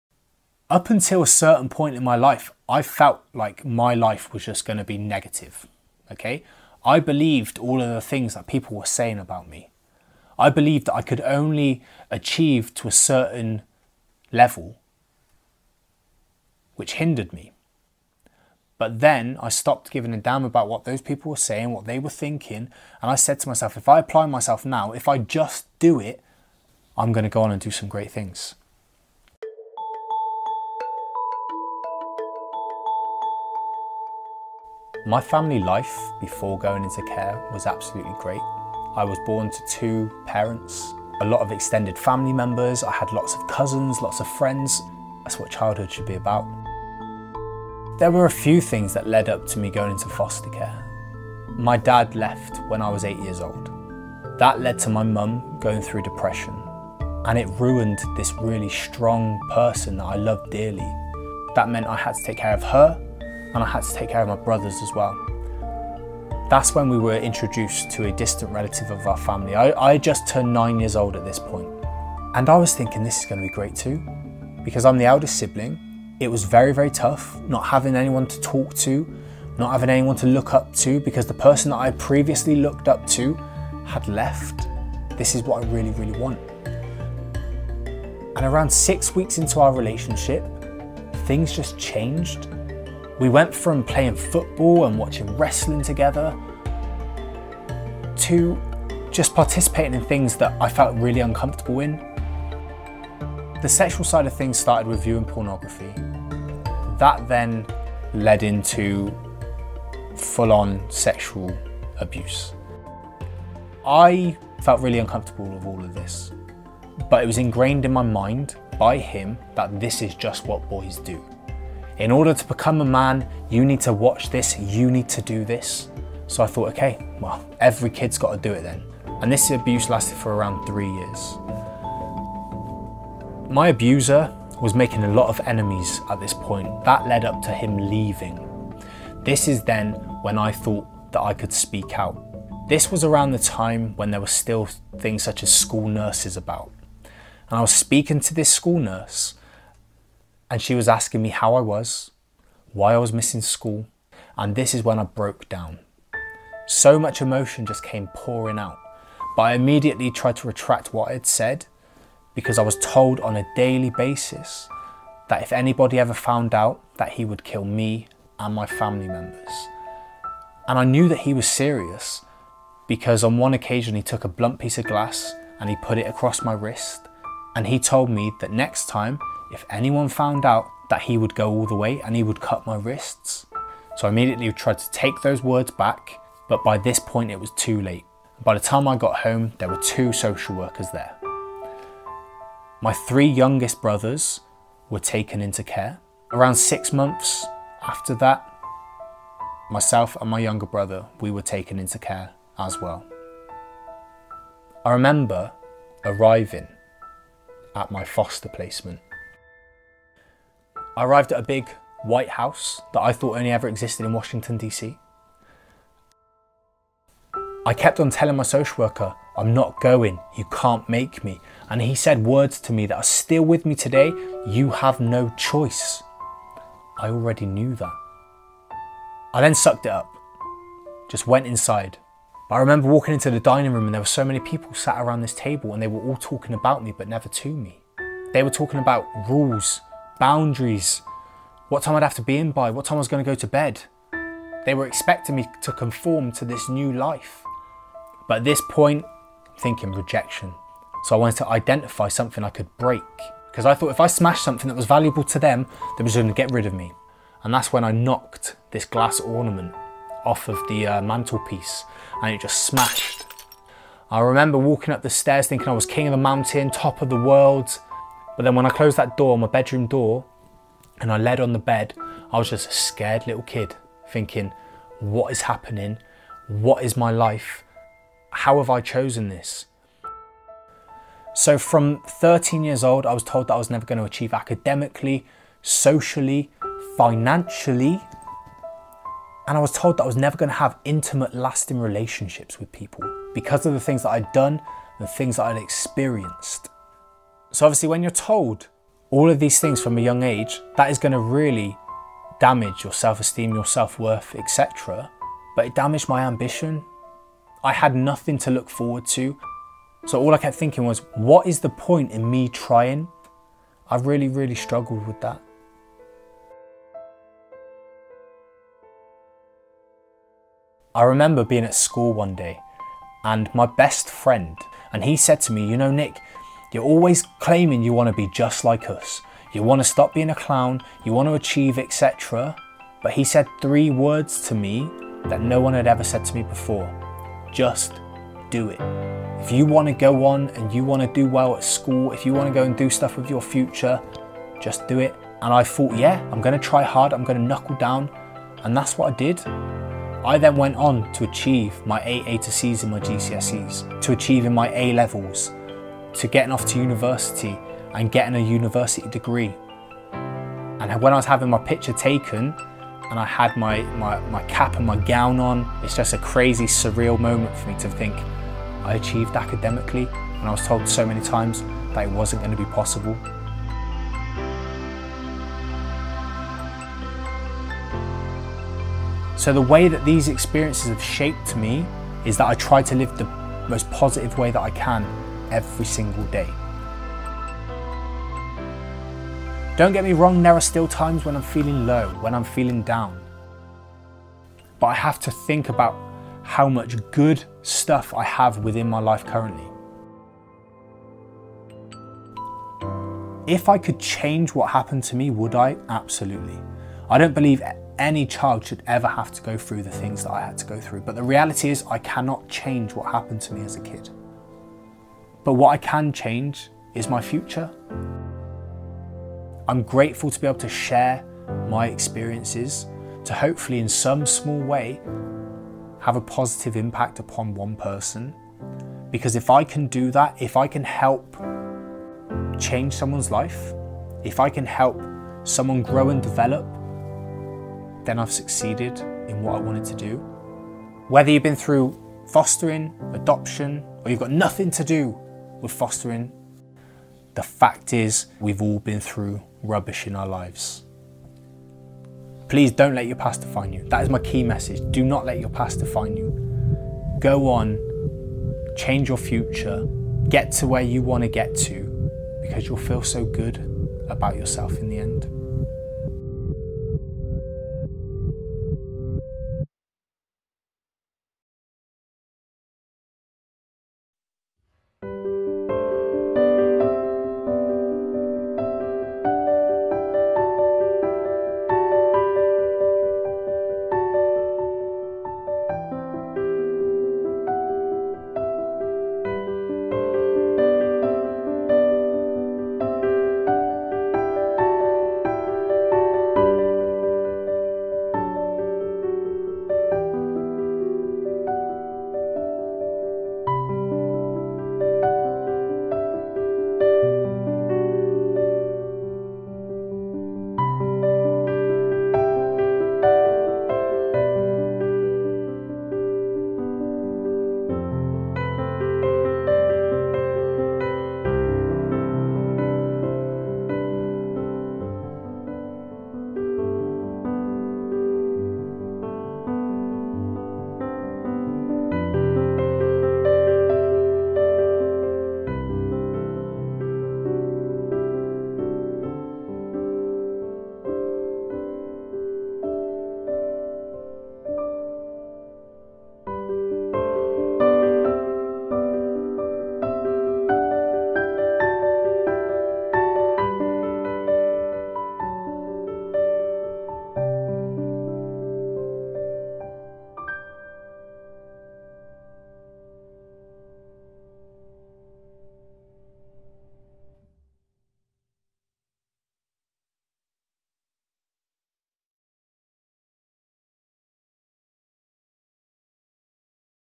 Nick Barwick on Overcoming Adversity
A short documentary about Nick Barwick, care leaver and motivational speaker. After going through all manner of trials, he overcame it all to become a success story.

Told with the unique style of silhouette stop-motion animation. Animated and Directed by Ryan Potter